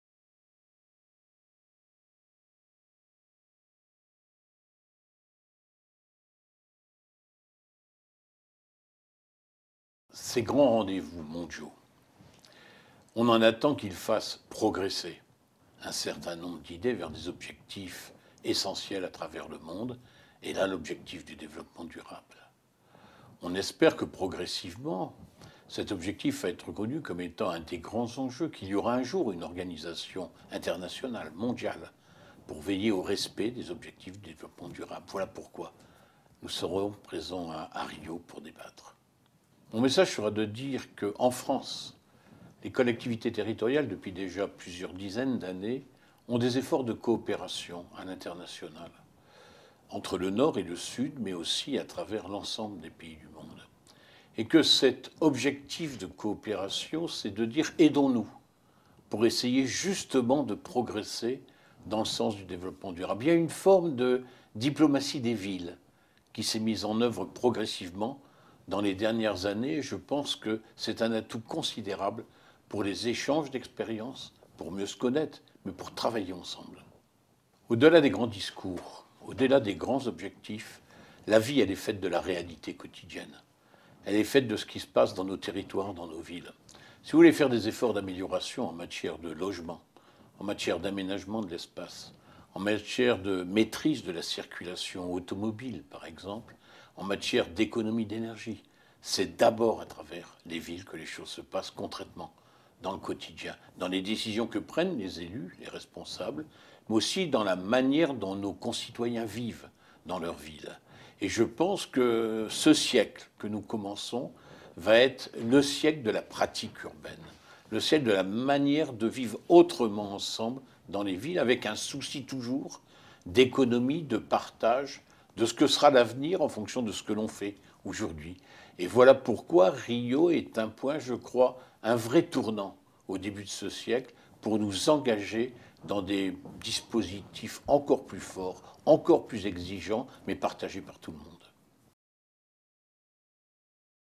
Rio+20 : Villes et territoires - Juin 2012
Rio+20 vu par Michel Delebarre, Président de Cités Unies France.
Du 20 au 22 juin 2012, une Conférence des Nations unies sur le développement durable se tiendra à Rio. 20 ans après le Sommet de la Terre, les dirigeants du monde entier se retrouvent une nouvelle fois au Brésil pour prendre des engagements décisifs pour le développement durable et, au delà, pour l'avenir de la planète.

Instragram : @francediplo